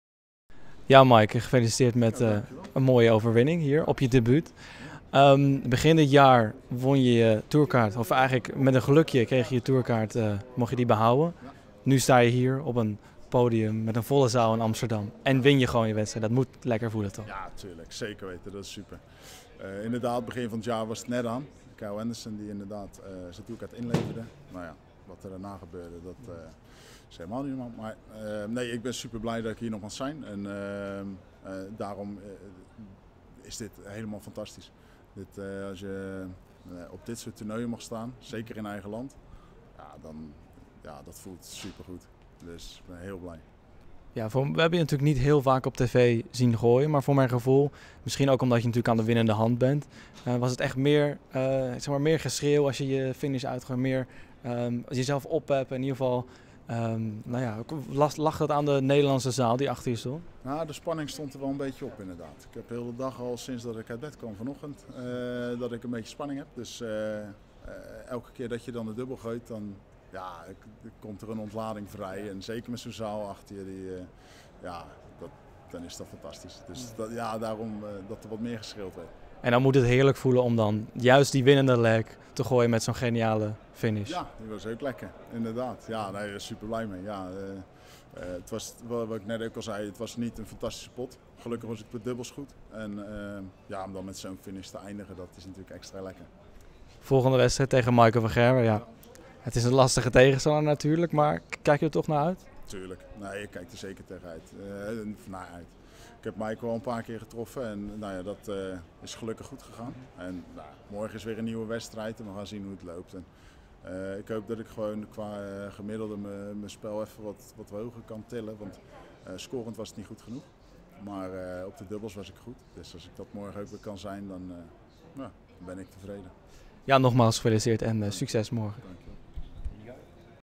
Maik Kuivenhoven: 'De Nederlandse zaal achter je is fantastisch' | World Series Finals 2021
Dit weekend wordt de World Series of Darts Finals gespeeld in AFAS Live te Amsterdam. Maik Kuivenhoven won afgelopen vrijdagavond met 6-2 van John Henderson en plaatste zich hiermee voor de tweede ronde. Na afloop sprak Darts Actueel met hem!

Al het dartnieuws waaronder de World Series of Darts Finals is terug te vinden op onze website!